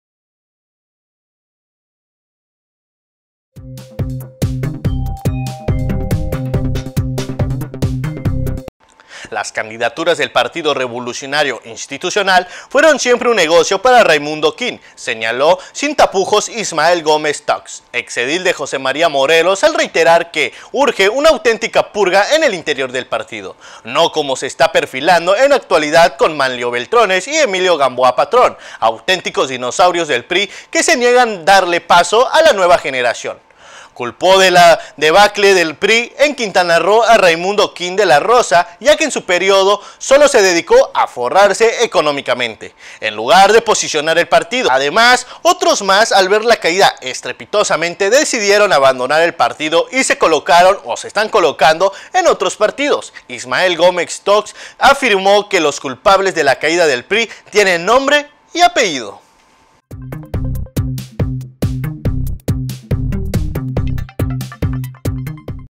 LOS CULPABLES DE LA CAIDA DEL PRI TIENEN NOMBRE Y APELLIDO: GÓMEZ TOX
Entérate de las noticias “EN CORTO”…LOS CULPABLES DE LA CAIDA DEL PRI TIENEN NOMBRE Y APELLIDO: GÓMEZ TOX...